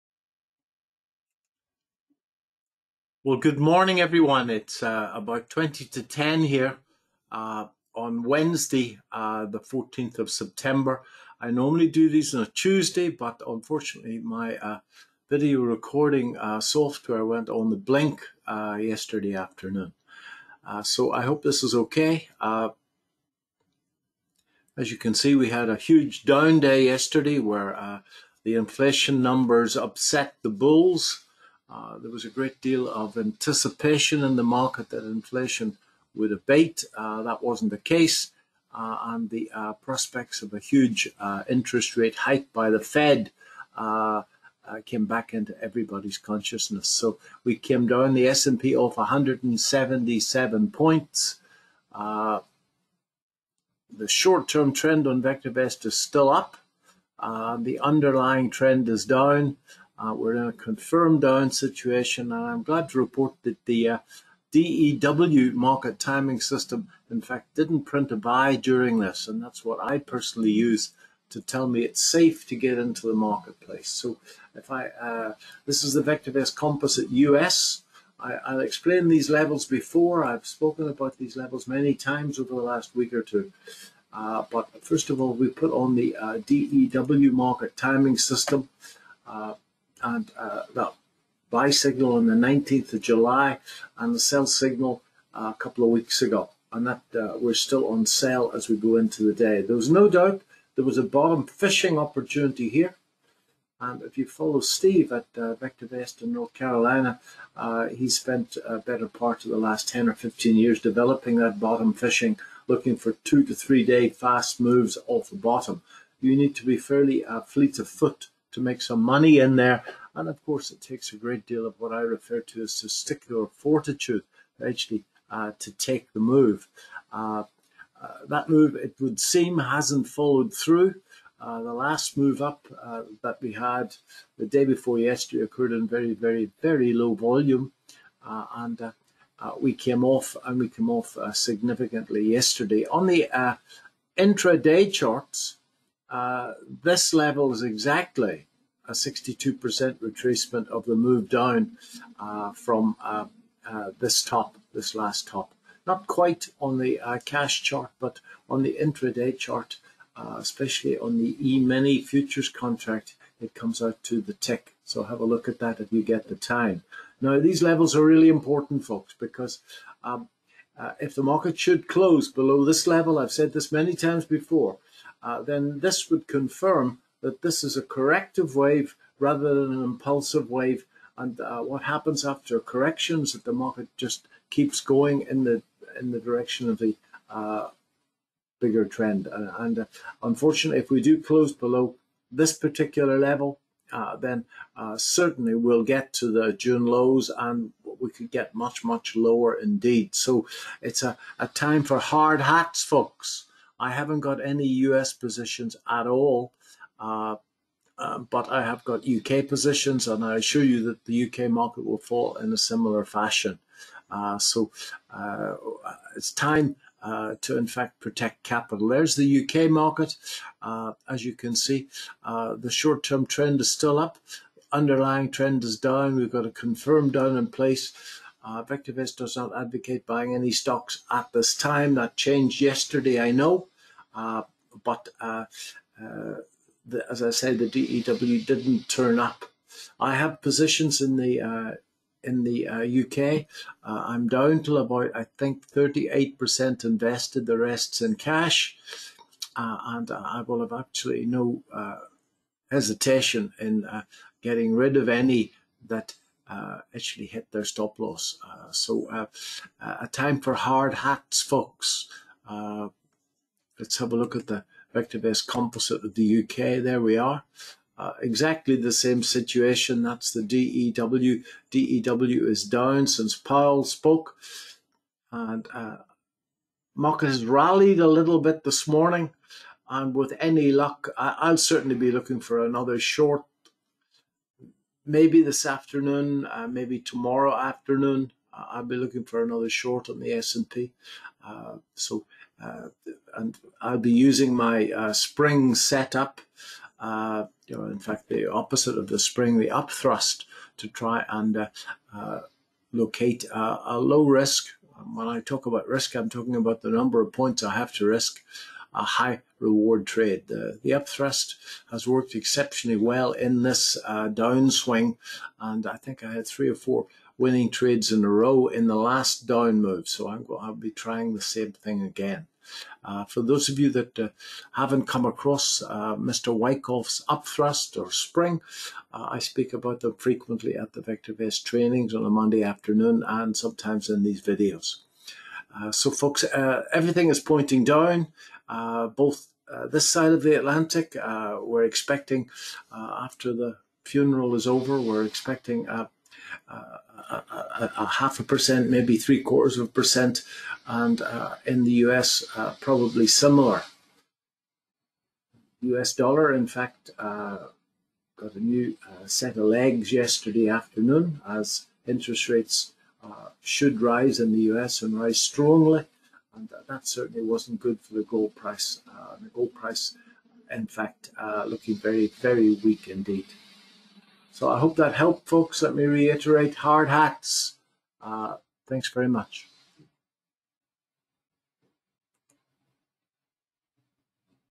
Hard hats and steel toecap boots required
The inflation number disappointed and the bulls ran for cover on the 13th of September 2022. The disappointing number infers a large rate hike by the FED. Stocks tumbled while the dollar gained across the board. In the video David points out a significant level which if breached will precipitate a further strong selloff. Please protect capital.